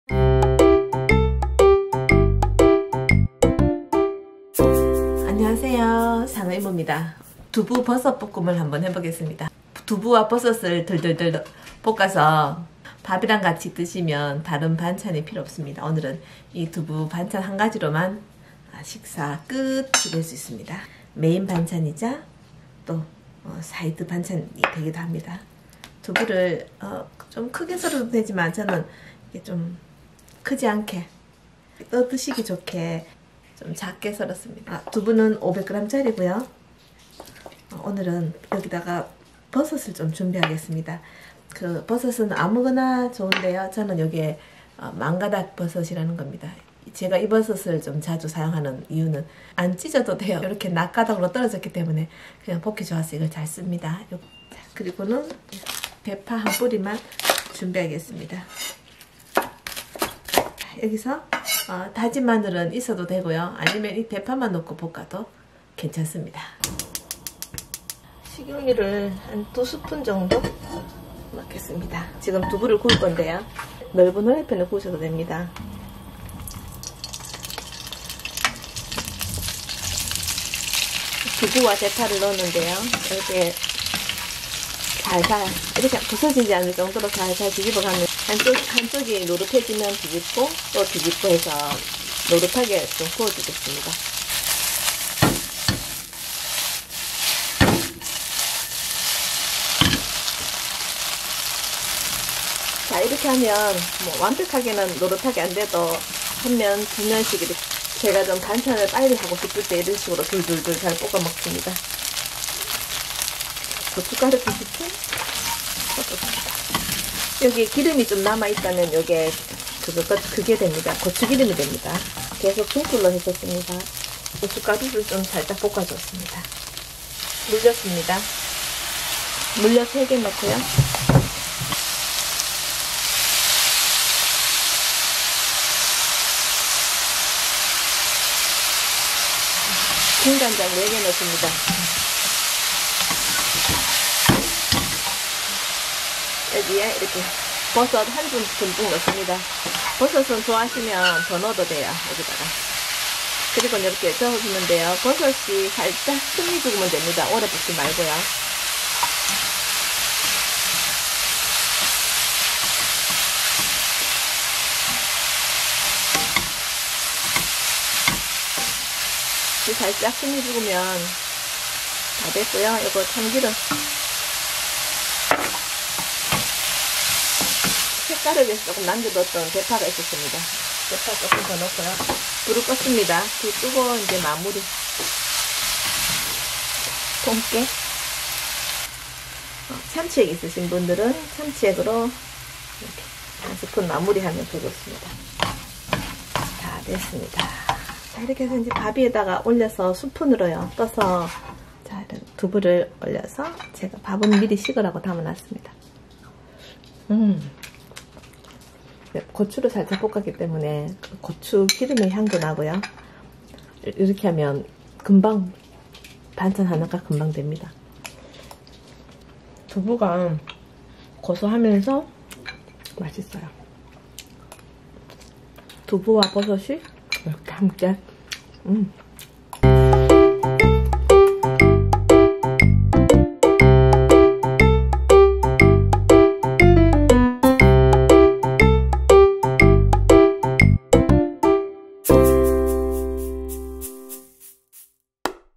두부볶음 간단하게 두부와 버섯으로 들들볶아서 한끼반찬 해결[상어이모.SANGEOIMO]
두부 500g(큰것)
버섯1줌